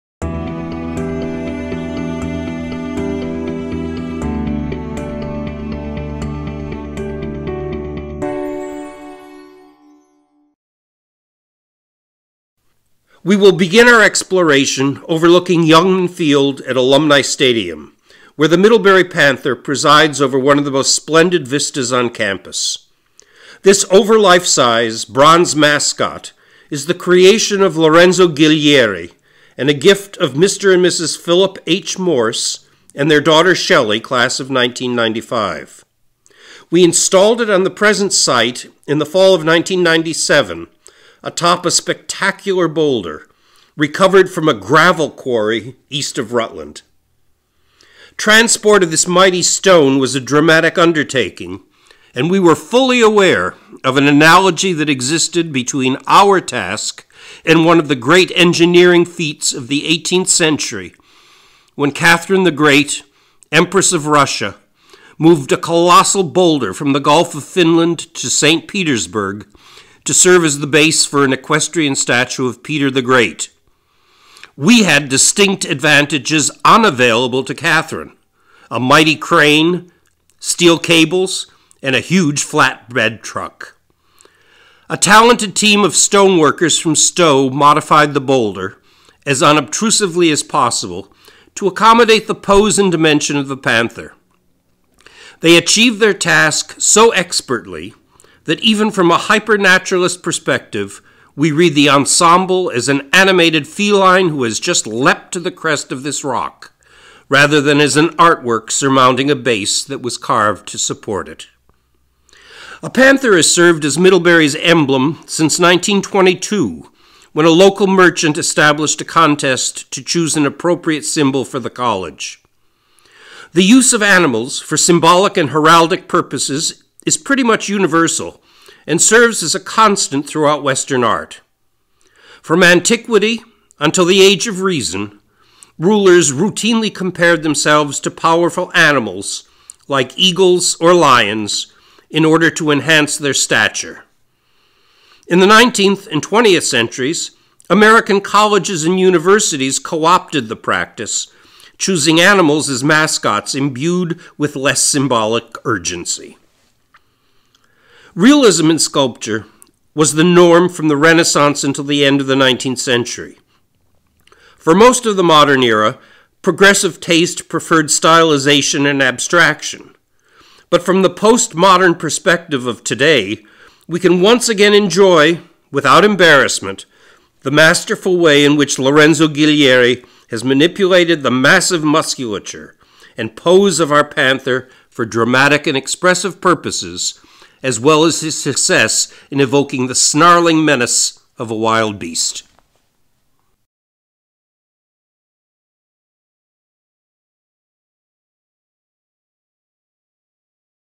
Middlebury College's Public Art Collection - Lorenzo Ghiglieri
The Committee on Art in Public Places (CAPP) at Middlebury College oversees a campus-wide collection of 19 works of public art. John Hunisak, professor of history of art and architecture at Middlebury and original chair of the committee, shares here a digital version of his popular walking tour of the collection. This installment highlights the Middlebury Panther by Lorenzo Ghiglieri.